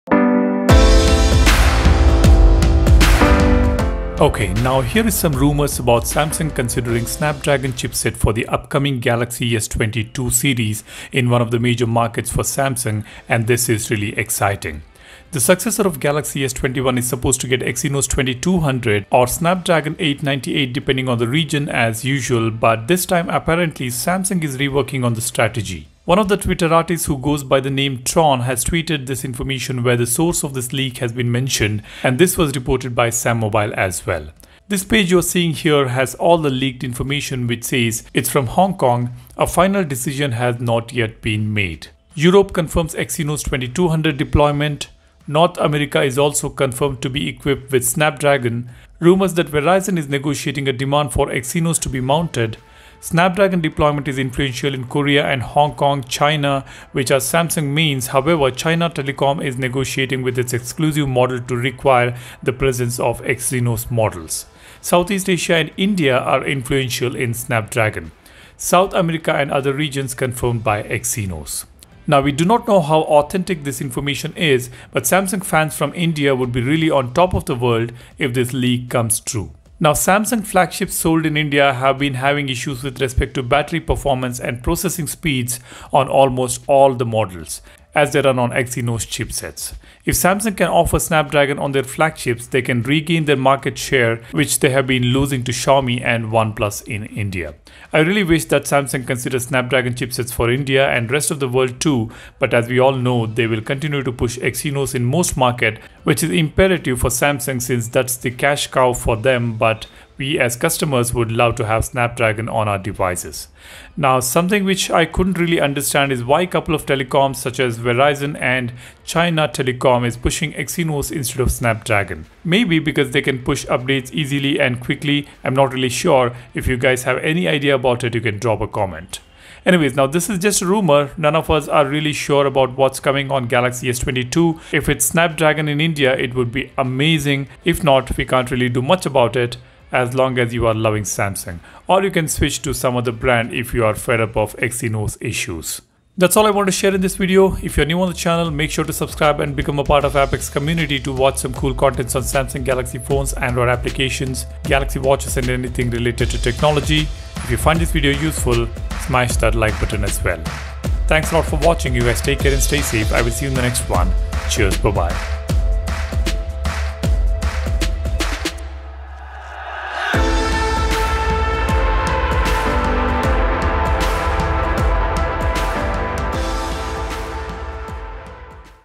Samsung Galaxy S 22 - Snapdragon is expected in India?
Here is some leaks about the upcoming Samsung galaxy S22 which claims that India may get Snapdragon 898 instead of Exynos 2200.